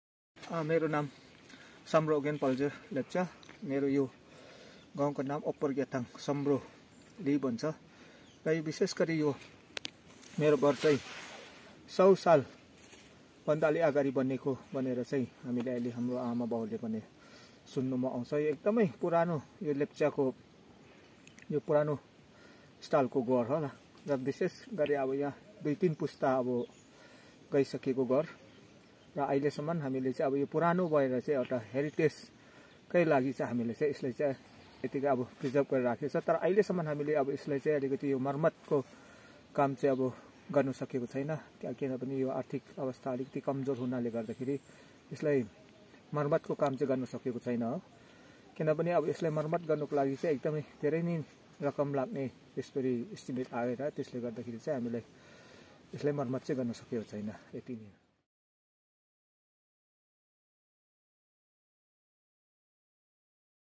PRA at Hee Gyathang ICAR-NEH Dzongu Government of Sikkim
Visiting one of the oldest traditional Lepcha house at Hee Gyathang, Dzongu, North Sikkim.